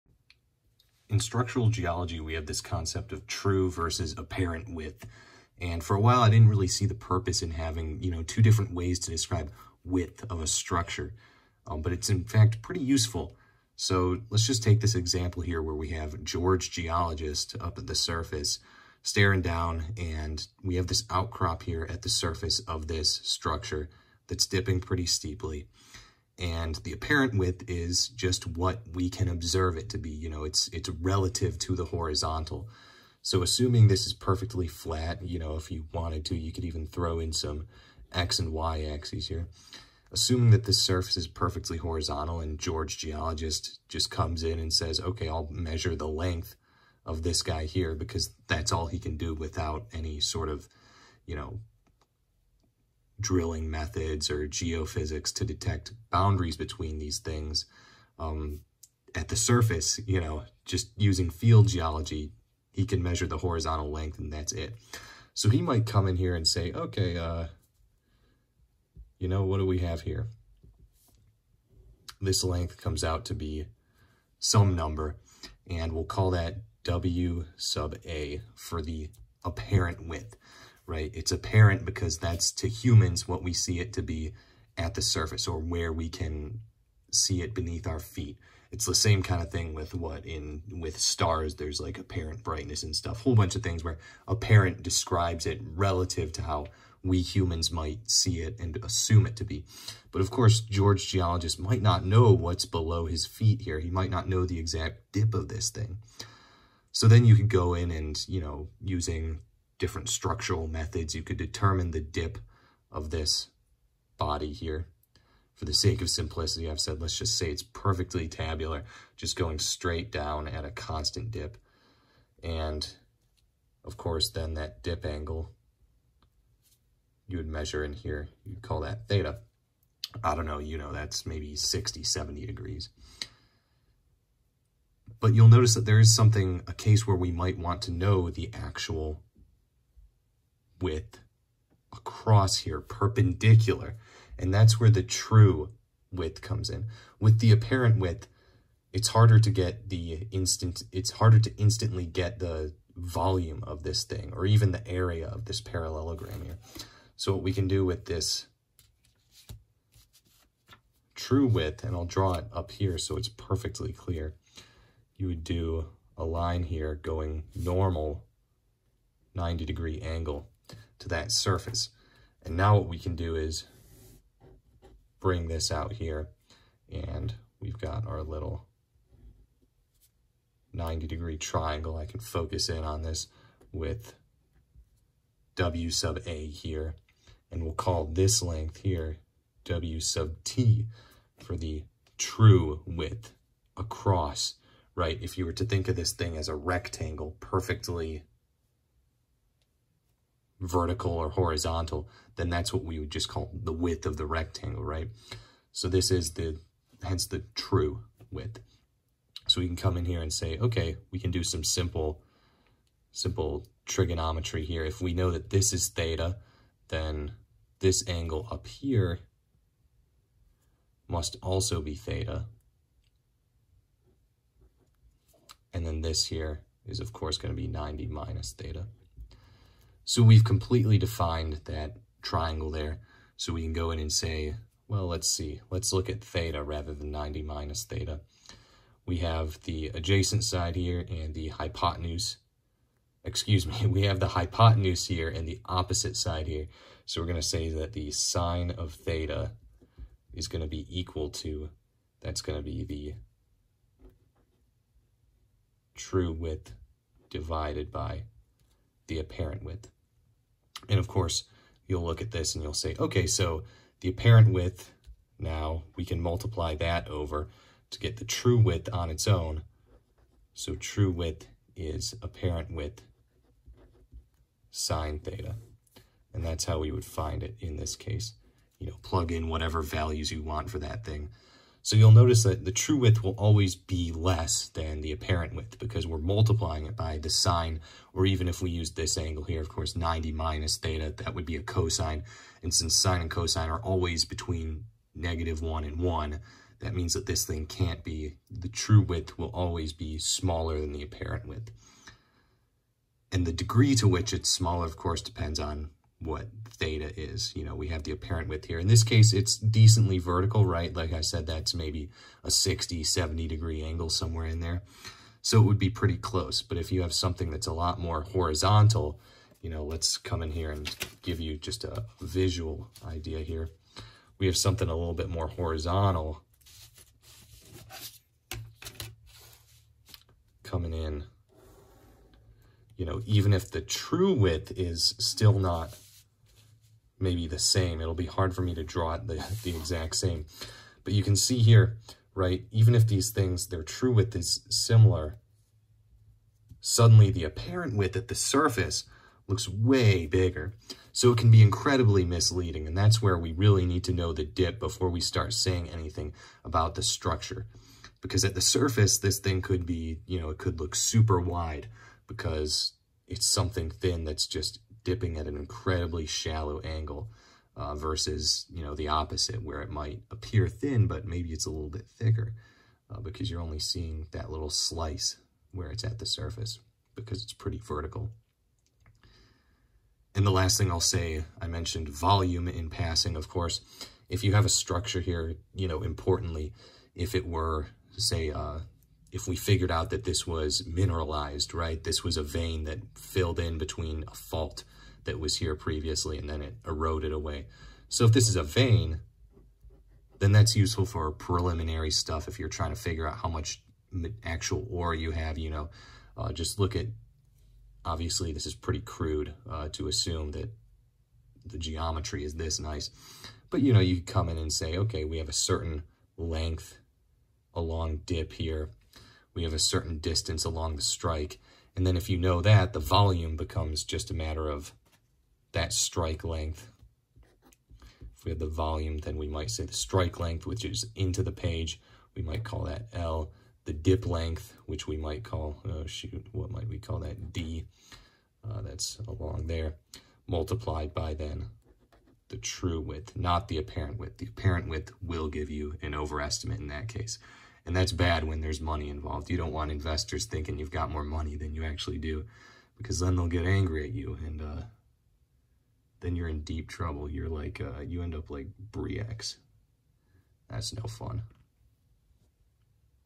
True and Apparent Width
A discussion of why we have two different ways of measuring width, and which one may be more useful.